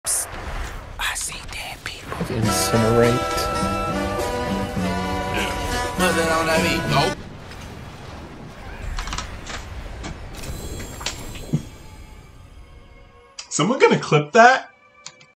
Incinerate is the Strongest Skill in Season 4
With the new aspect, you can have 3 streams of 170% damage, and they shotgun.